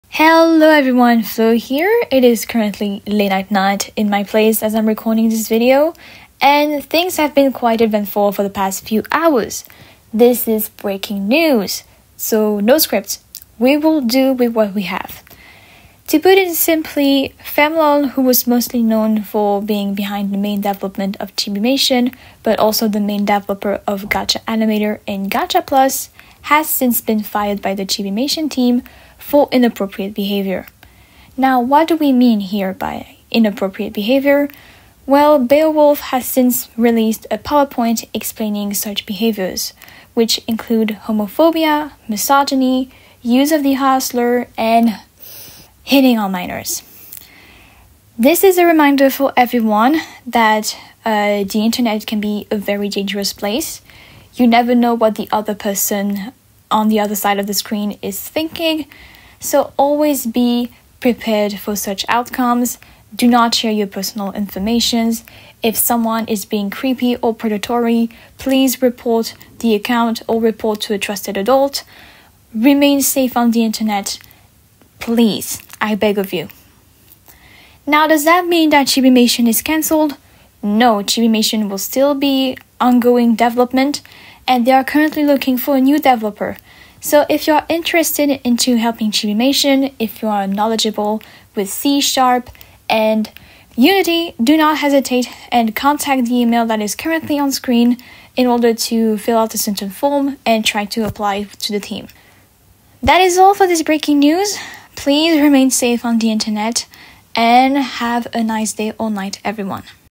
[TW] ChibiMation fires developer because of his JAW DROPPING behaviour
Don’t hesitate to pause the video, and if interested, here’s the link to the PowerPoint

Credits:
Vtuber Model and set up
- Koyuki (Art)
- Upxie (Rig)
- vexxox0 [Instagram] (logo)